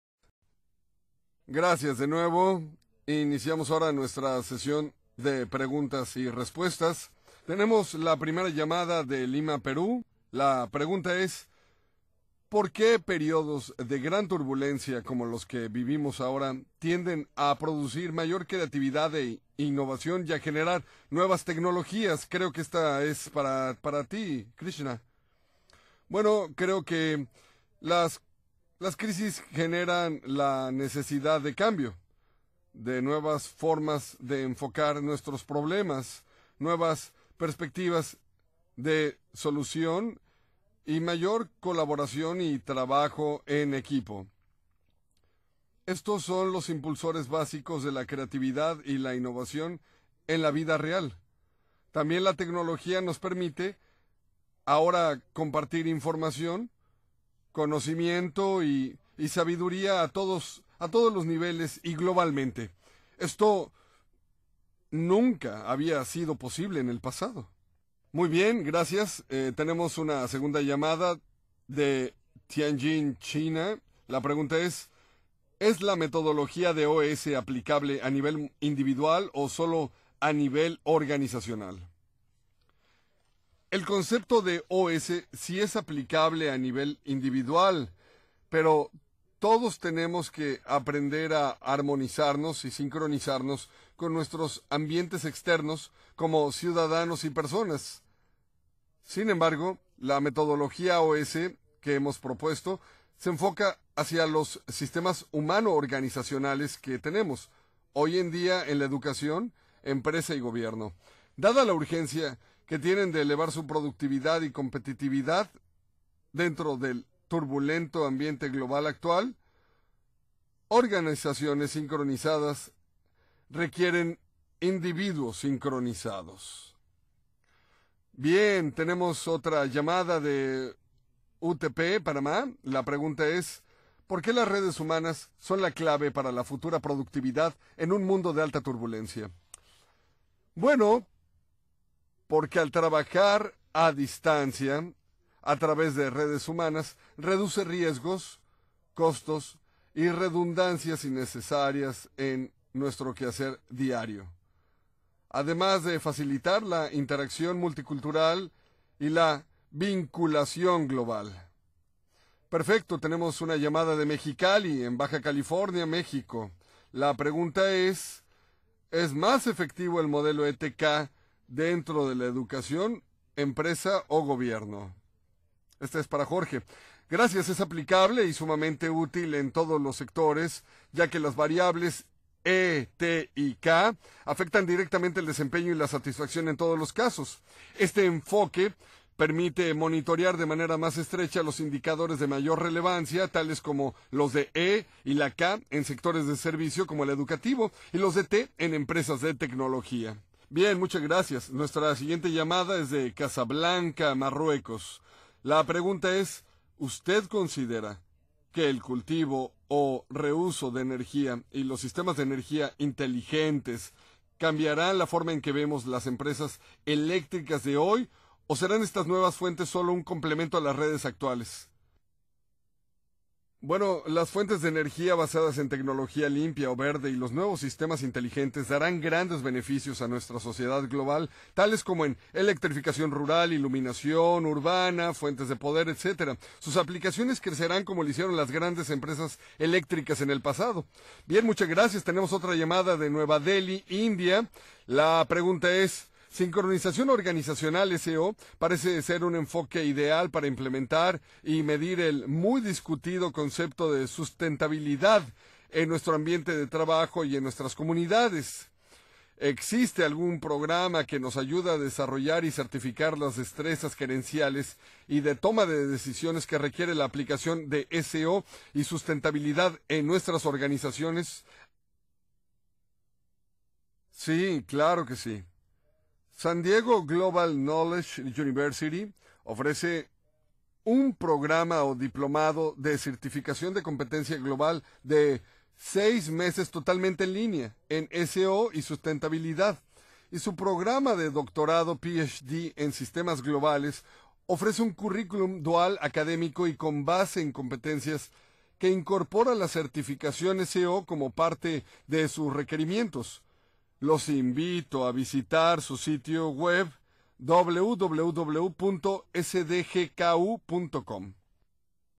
Sincronizando Organizaciones a la Turbulencia Global del Siglo 21: Estrategias y Mercados (Parte 4)
Introducción

Sincronizando Organizaciones a la Turbulencia Global del Siglo 21: Estrategias y Mercados

Presentador: M. S. Krishnamoorthy

Es una realidad que el cambio y la turbulencia global son las características distintivas del Siglo XXI. Las organizaciones que no adapten y armonicen sus estrategias, tácticas y operaciones a esta realidad fracasarán a mediano y largo plazo. El proceso de globalización, impulsado por una explosión demográfica reactivada y por el surgimiento de la Nueva Economía, está obligando a educadores, líderes empresariales, gobierno y sociedad civil por todo el mundo a re-pensar los enfoques pasados hacia la productividad y el éxito de mercado que no incluían una perspectiva global y una respuesta eficaz a las incertidumbres y riesgos de un ambiente que ahora exhibe constante conflicto e inestabilidad. Las grandes oportunidades de mercado que ofrece ahora la explosión del nuevo conocimiento y las nuevas tecnologías de información estarán al alcance solo de quienes tengan éxito en alinear y sincronizar sus visiones, recursos y acciones a esta nueva realidad.

Esta importante videoconferencia repasará los conceptos de turbulencia global y sincronización organizacional en relación a la productividad y la competitividad. Los distinguidos expositores invitados hablarán sobre el surgimiento de nuevos líderes globales tales como Nokia (Finlandia) en el ramo de diseño de aparatos móviles de comunicación y la industria de cine de la India, ahora la más grande del mundo, como una creciente tendencia de multi-polaridad cultural y competencia que está obligando a todos los competidores a buscar nuevas alianzas y asociaciones, adaptándose a nuevos valores, clientes y preferencias de consumo. La estrategia y práctica de los negocios debe ahora adoptar nuevas reglas de actuación colaborativa en un ambiente global de cambiante dinámica e interacción cultural, tecnológica y del conocimiento. El desarrollo de destrezas y competencias globales de liderazgo se convierte así en una urgente necesidad para la sustentabilidad y el éxito productivo.